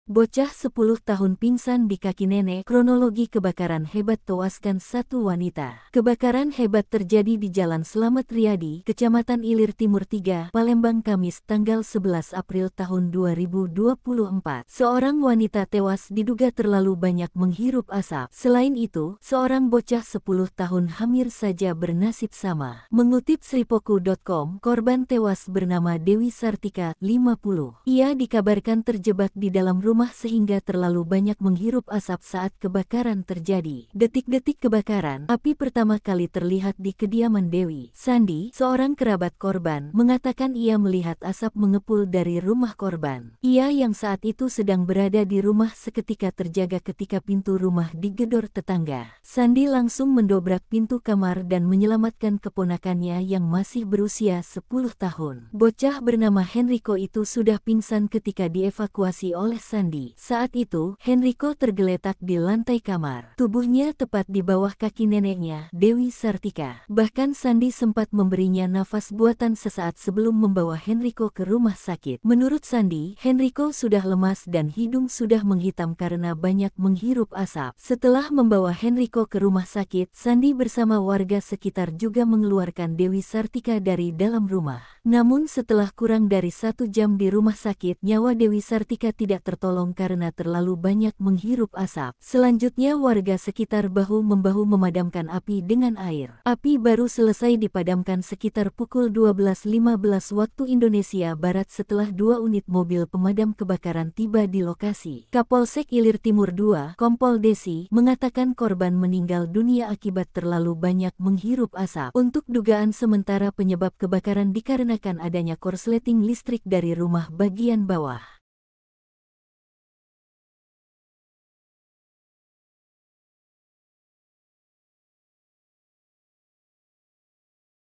Bocah 10 Tahun Pingsan di Kaki Nenek, Kronologi Kebakaran Hebat Tewaskan Satu Wanita
Kebakaran hebat terjadi di Jalan Slamet Riady, Kecamatan Ilir Timur III, Palembang Kamis (11/4/2024).

Seorang wanita tewas diduga terlalu banyak menghirup asap.

Selain itu, seorang bocah 10 tahun hamir saja bernasib sama.

Palembang.tribunnews.com

Sumber Video: localpridegarage
Editor Video: frizeky
Vo: felin